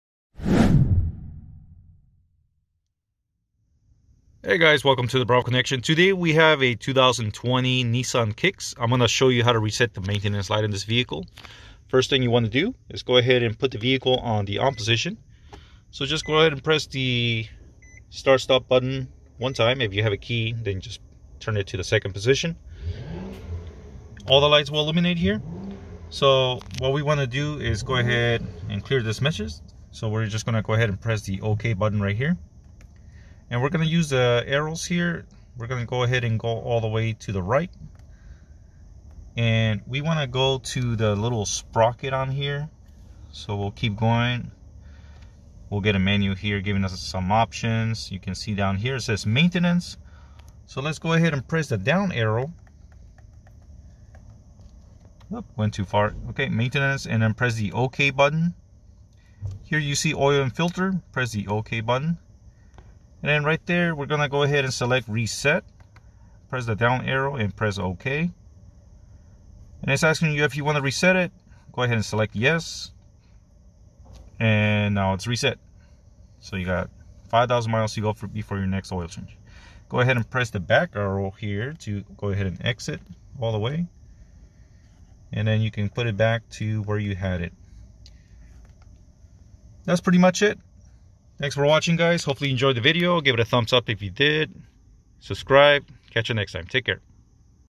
2018 - 2021 Nissan Kicks oil light maintenance reset
how to reset the maintenance oil light after an oil change.
works on 2018 , 2019, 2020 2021